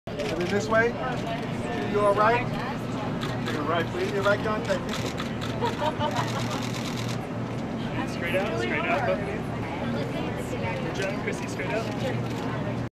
Chrissy Teigen and John Legend at Benefit Cosmetics NYFW ...
Chrissy Teigen and John Legend at Benefit Cosmetics NYFW They're REAL-volutionary Awards on September 04, 2013 in New York, New York